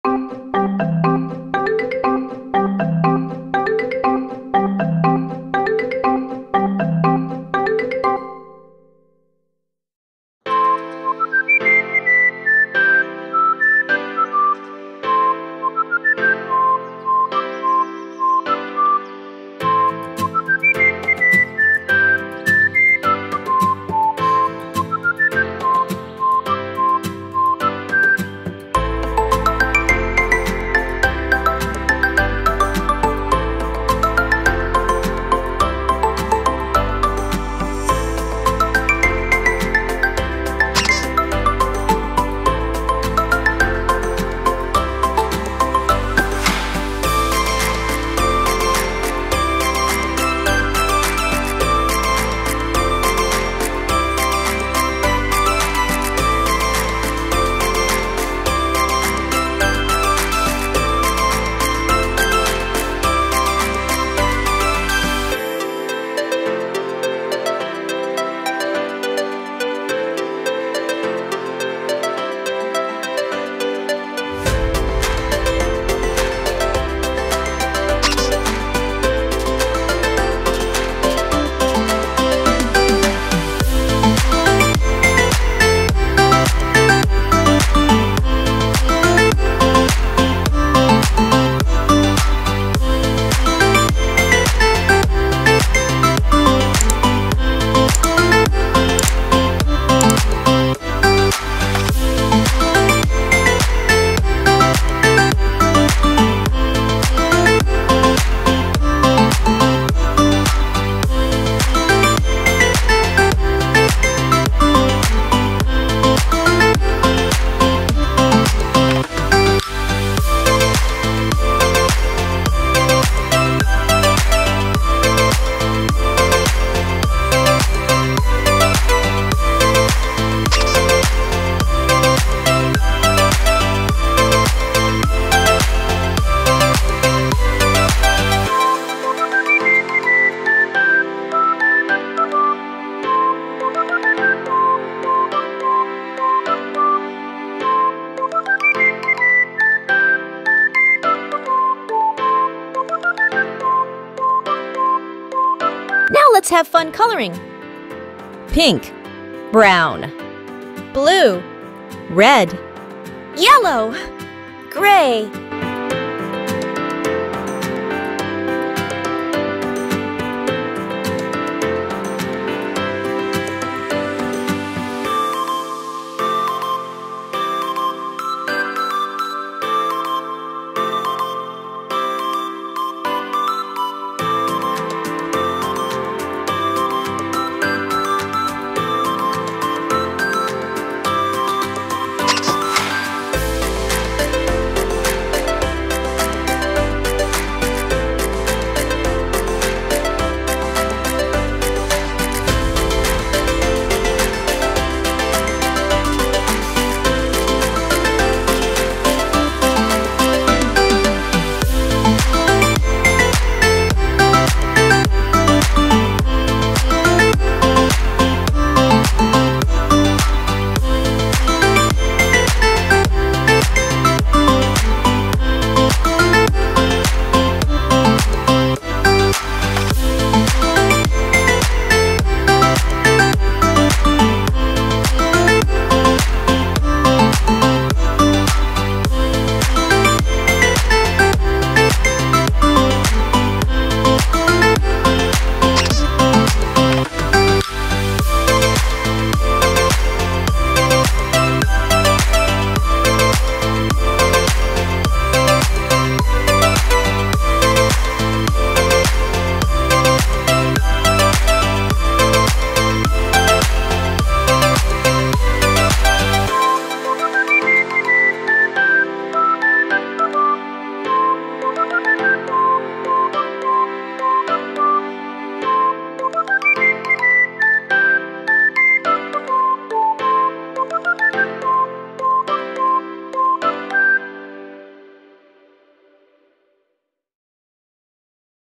Toilet Drawing, Painting and coloring for kids |Draw, Paint and Learn
In this video, we're going to draw a Toilet. Follow along with me and your kids can learn how to draw these things, too! Let's get started.

The drawing tutorial covers,
1. Step by step drawing
2. Easy for kids
3. Theme friendly for kids
4. drawing for kids
5. Toilet drawing for kids